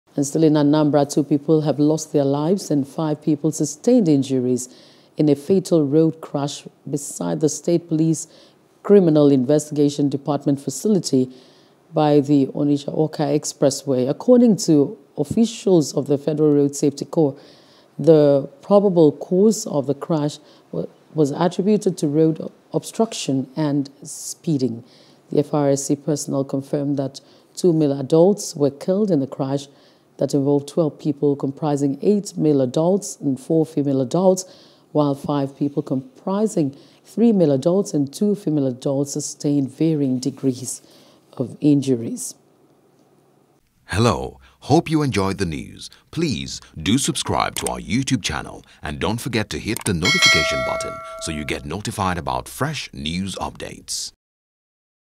Anambra: Two Dead, Five Injured In Road Crash | NEWS
Two people have lost their lives and five people sustained injuries in a fatal road crash beside the State Police Criminal Investigation Department facility, by the Onitsha-Awka Expressway.